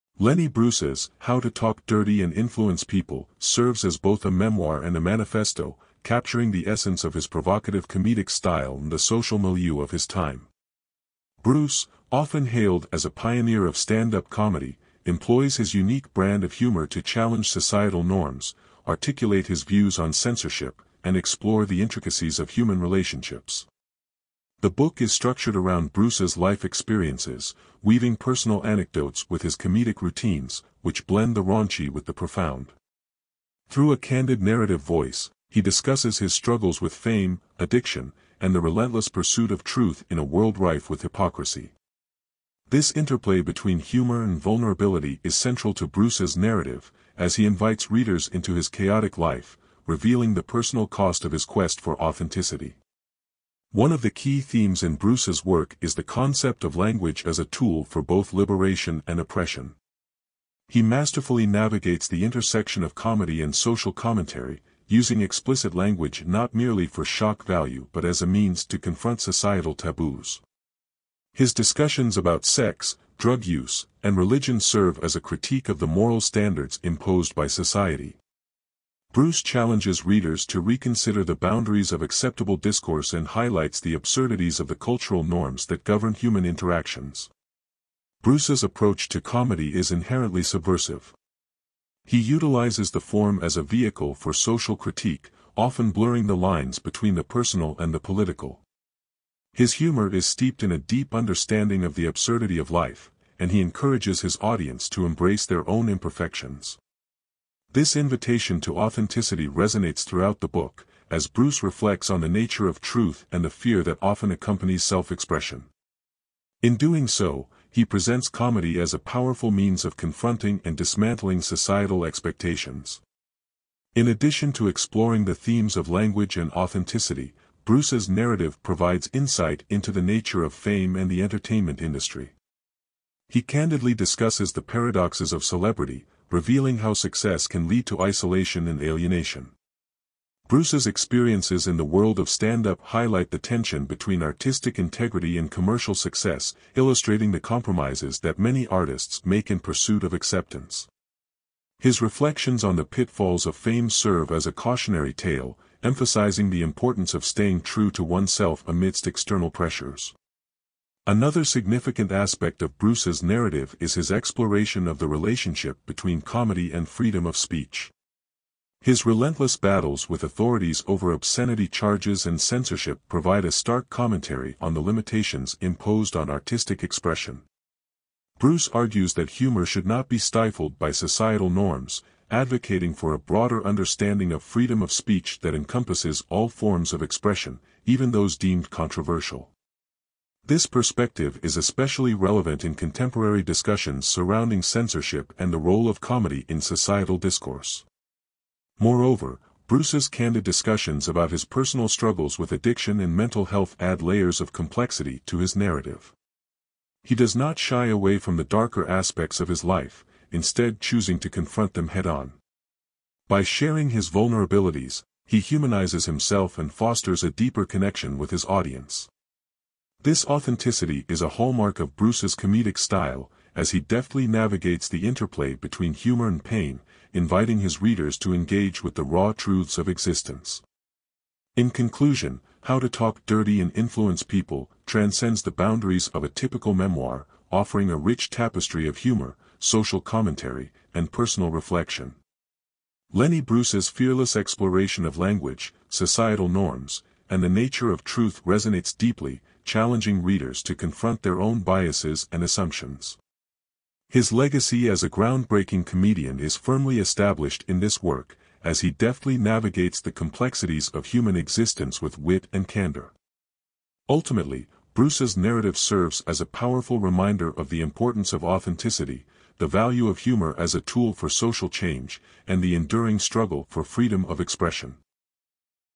"How To Talk Dirty And Influence People" By Lenny Bruce
Lenny Bruce's "How to Talk Dirty and Influence People" serves as both a memoir and a manifesto, capturing the essence of his provocative comedic style and the social milieu of his time. Bruce, often hailed as a pioneer of stand-up comedy, employs his unique brand of humor to challenge societal norms, articulate his views on censorship, and explore the intricacies of human relationships.The book is structured around Bruce's life experiences, weaving personal anecdotes with his comedic routines, which blend the raunchy with the profound. Through a candid narrative voice, he discusses his struggles with fame, addiction, and the relentless pursuit of truth in a world rife with hypocrisy. This interplay between humor and vulnerability is central to Bruce's narrative, as he invites readers into his chaotic life, revealing the personal cost of his quest for authenticity.One of the key themes in Bruce's work is the concept of language as a tool for both liberation and oppression. He masterfully navigates the intersection of comedy and social commentary, using explicit language not merely for shock value but as a means to confront societal taboos. His discussions about sex, drug use, and religion serve as a critique of the moral standards imposed by society. Bruce challenges readers to reconsider the boundaries of acceptable discourse and highlights the absurdities of the cultural norms that govern human interactions.Bruce's approach to comedy is inherently subversive. He utilizes the form as a vehicle for social critique, often blurring the lines between the personal and the political. His humor is steeped in a deep understanding of the absurdity of life, and he encourages his audience to embrace their own imperfections. This invitation to authenticity resonates throughout the book, as Bruce reflects on the nature of truth and the fear that often accompanies self-expression. In doing so, he presents comedy as a powerful means of confronting and dismantling societal expectations.In addition to exploring the themes of language and authenticity, Bruce's narrative provides insight into the nature of fame and the entertainment industry. He candidly discusses the paradoxes of celebrity, revealing how success can lead to isolation and alienation. Bruce’s experiences in the world of stand-up highlight the tension between artistic integrity and commercial success, illustrating the compromises that many artists make in pursuit of acceptance. His reflections on the pitfalls of fame serve as a cautionary tale, emphasizing the importance of staying true to oneself amidst external pressures.Another significant aspect of Bruce's narrative is his exploration of the relationship between comedy and freedom of speech. His relentless battles with authorities over obscenity charges and censorship provide a stark commentary on the limitations imposed on artistic expression. Bruce argues that humor should not be stifled by societal norms, advocating for a broader understanding of freedom of speech that encompasses all forms of expression, even those deemed controversial. This perspective is especially relevant in contemporary discussions surrounding censorship and the role of comedy in societal discourse.Moreover, Bruce's candid discussions about his personal struggles with addiction and mental health add layers of complexity to his narrative. He does not shy away from the darker aspects of his life, instead choosing to confront them head-on. By sharing his vulnerabilities, he humanizes himself and fosters a deeper connection with his audience. This authenticity is a hallmark of Bruce's comedic style, as he deftly navigates the interplay between humor and pain, inviting his readers to engage with the raw truths of existence.In conclusion, "How to Talk Dirty and Influence People" transcends the boundaries of a typical memoir, offering a rich tapestry of humor, social commentary, and personal reflection. Lenny Bruce's fearless exploration of language, societal norms, and the nature of truth resonates deeply, challenging readers to confront their own biases and assumptions. His legacy as a groundbreaking comedian is firmly established in this work, as he deftly navigates the complexities of human existence with wit and candor. Ultimately, Bruce's narrative serves as a powerful reminder of the importance of authenticity, the value of humor as a tool for social change, and the enduring struggle for freedom of expression.